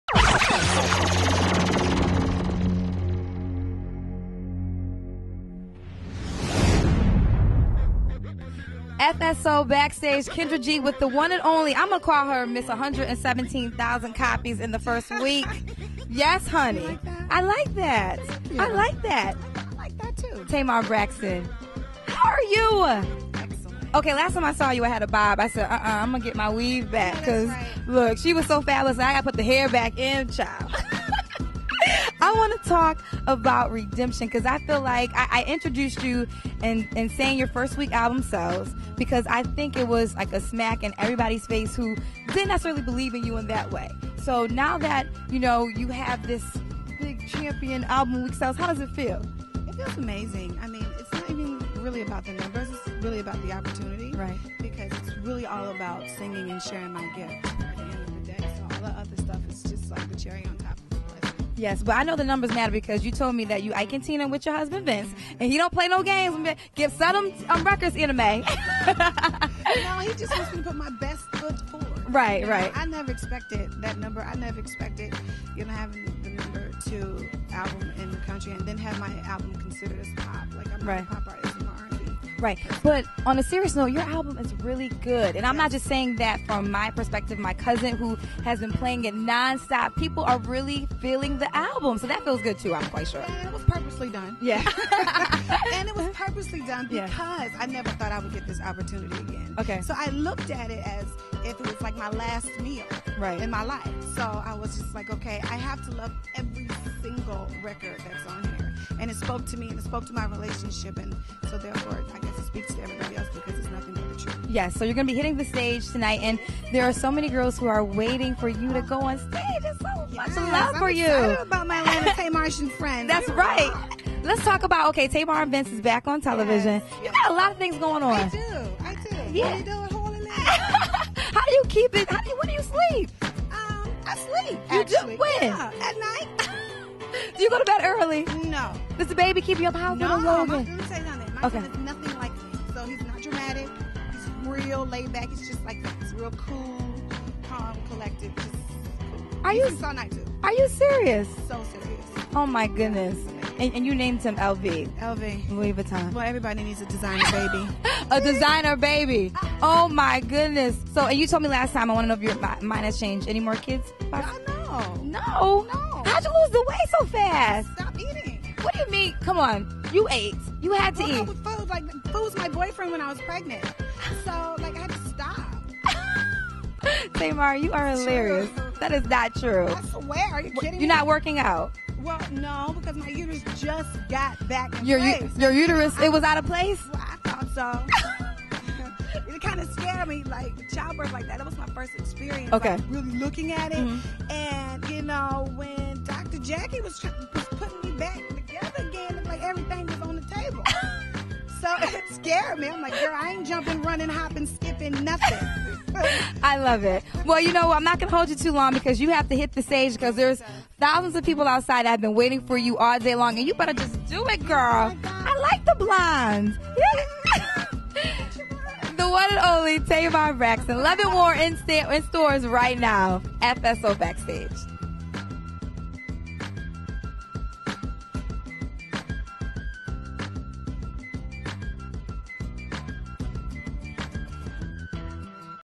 Tamar Braxton And Kendra G At V-103 FSO 2013
V-103's Kendra G interviewing Tamar Braxton at FSO 2013.